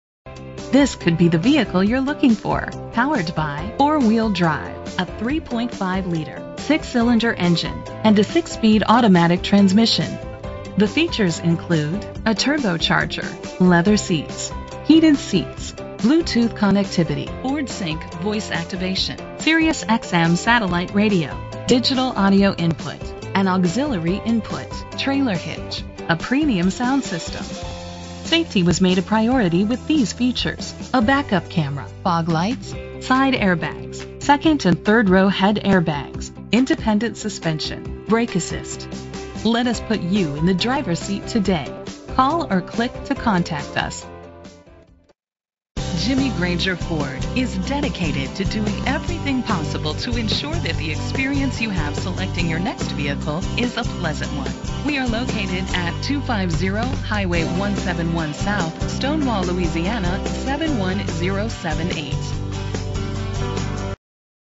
2015 Ford Explorer - Stonewall LA
Phone:
Year: 2015
Make: Ford
Model: Explorer
Trim: Sport
Engine: 3.5 liter 6 cylinder 24 valve
Transmission: 6-Speed Automatic
Color: Tuxedo Black Metallic
Mileage: 0
Address:
251 Highway 171 South